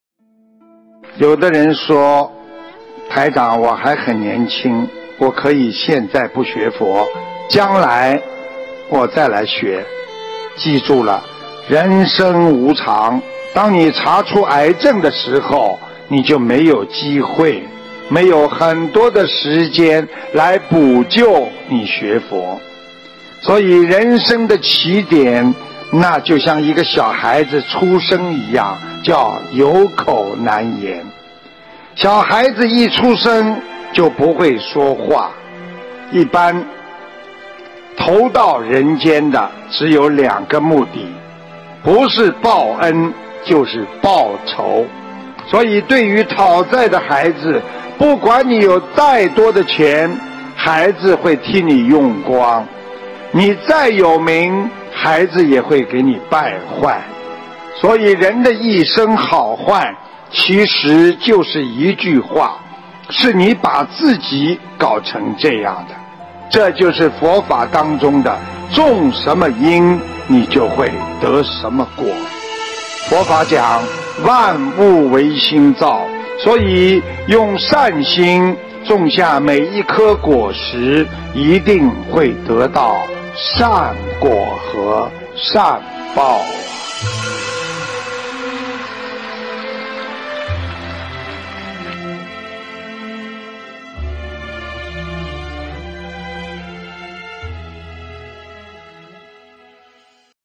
我还很年轻，我可以现在不学佛，将来我再来学 【 卢台长 观世音菩萨 心灵法门】
📫🌅有的人说：“师父，我还很年轻，我可以现在不学佛，将来我再来学。”
记住了，人生无常，当你查出癌症的时候，你就没有机会、没有很多时间来补救你学佛。

人生的起点，就像一个小孩子出生一样，有口难言。
小孩子一出生就不会说话，一般投到人间只有两个目的：不是报恩，就是报仇。

对于讨债的孩子，你有再多的钱，孩子会替你用光；你再有名，孩子也会给你败坏。人的一生好坏，其实就是一句话：是你把自己搞成这样的。这就是佛法当中的 “ 种什么因，你就会得什么果 ”。佛法讲万物唯心造，用善心种下每一颗果实，一定会得到善果和善报。

如果每一天种一点恶因，你到最后就是一个大恶人，你就自作自受。所以，孔老夫子讲过一句话
“不怨天，不尤人”，全是自己所为。
——2018年5月17日 新加坡开光日 开示